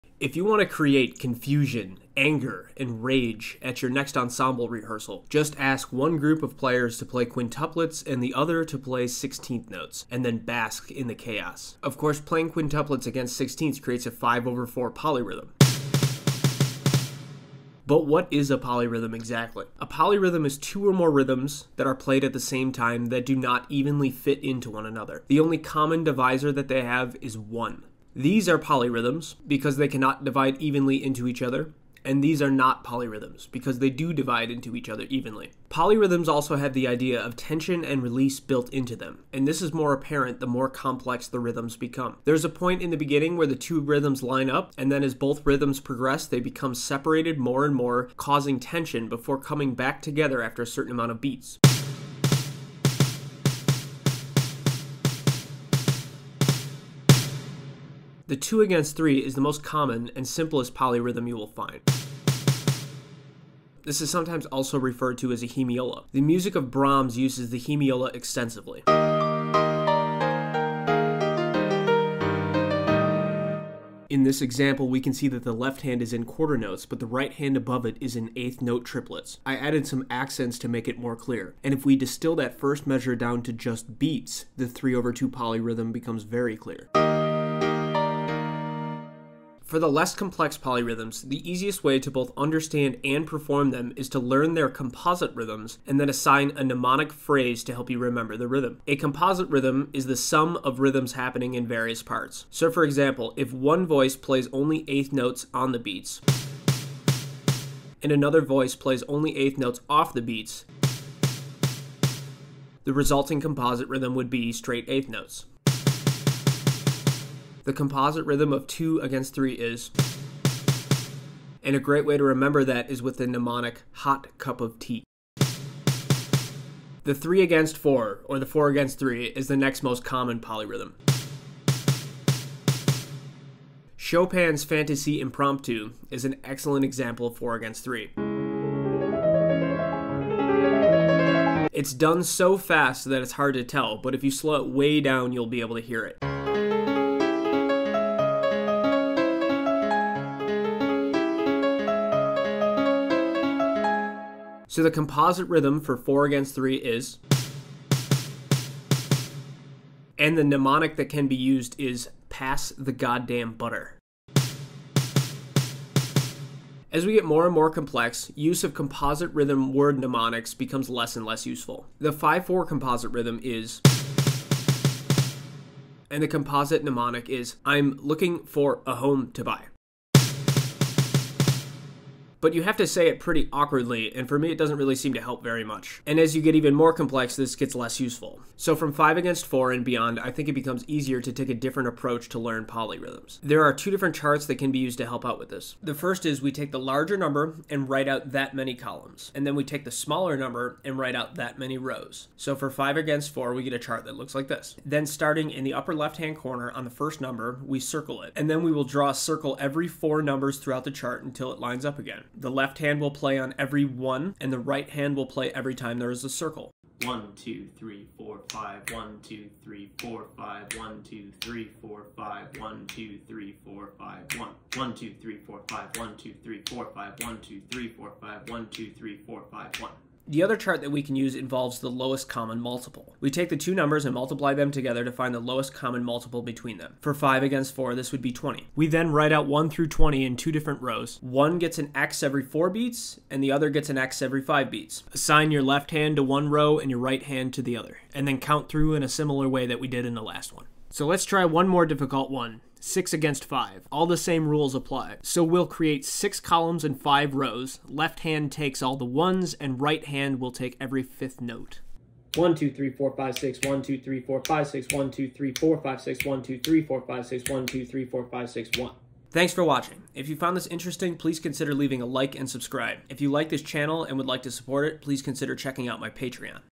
Polyrhythms in 5 Minutes or Less
Poly Rhythms in 5 Minutes or Less. In this video we dive deep into the basic poly rhythms, word mnemonics, composite rhythms and more advanced polyrhythms. We cover 3:2, 4:3, 5:4 and 6:5. Hope you enjoy!

0:00 what is a polyrhythm
0:58 2:3 Hemiola
1:29 composite rhythms
2:06 4:3
2:46 5:4
3:13 5:4 with another technique
4:10 6:5